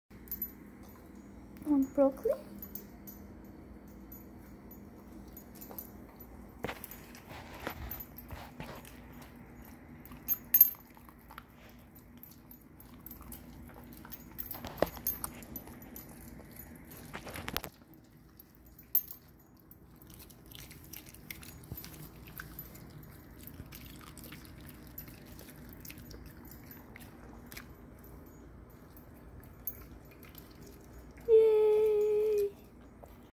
Sausage dog eats broccoli
Charlie the sausage in a new eating challenge